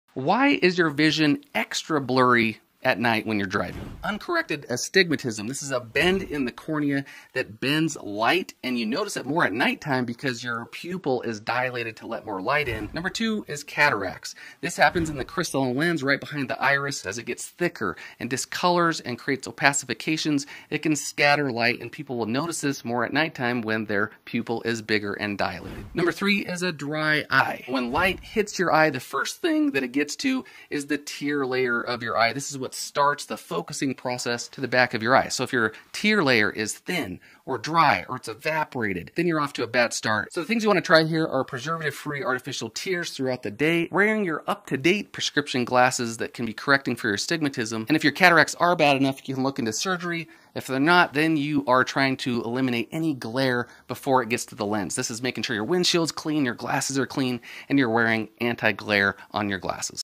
Why Is Your VIsion Blurry At Night?
Blurry vision at night is one of the most common symptoms people experience.

It is common for night vision to be extra blurry, including glare, halos, light streaks, and reduced contrast sensativity.  There can be many reasons for this, ranging from issues with glasses prescription to issues with dry eyes or possibly retina problems.  This video goes over many of the major issues that can cause night driving blur, and what some solutions can be.

INFO FROM FULL VIDEO (CLICK RELATED VIDEO TO WATCH)

1. Light From Image To Eye
-Clean Windshield
-Clean Glasses

2. Keep Glasses Prescription Up-To-Date
-Get your eye exam and up date your glasses prescription
-Night Myopia
-Get Anti-Reflective/Anti-Glare Coating on glasses
👉New Technology Anti-Glare Coating Glasses:
   Essilor Drive Lenses
   Zenni Driving Glasses

3. Dry Eyes and the Cornea

4. The Pupil

5. The Crystalline Lens (Cataracts)

6. Eye Floaters

7. Retina Problems
-Nyctalopia
-Retinitis Pigmentosa
-Vitamin A Deficiency

WHAT IS DISCUSSED IN THIS VIDEO

⚡Let’s Connect on Social Media!  Dr. Guymon (Dr. EyeGuy)⚡